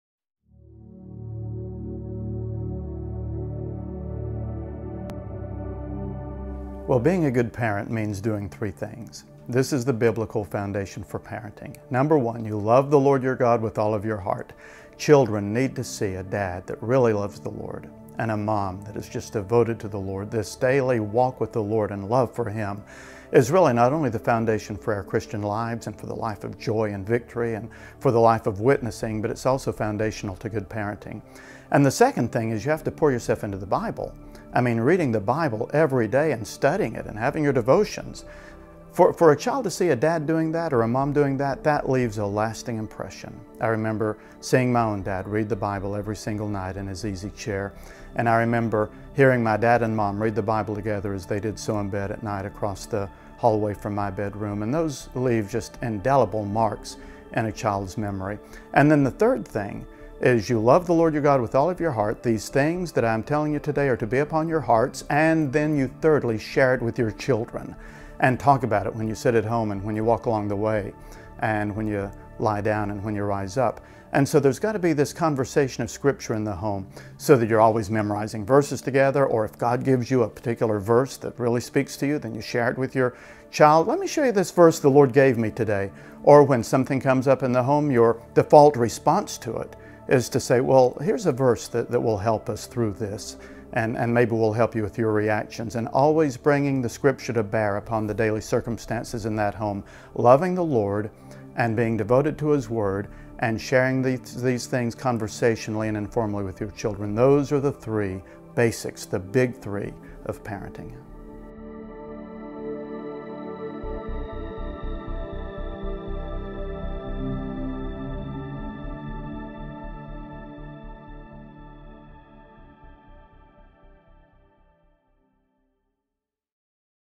What Does D6 Mean? [short]
Pastor, well-known speaker and best-selling author Rob Morgan discusses the meaning and significance of Deuteronomy 6:5-7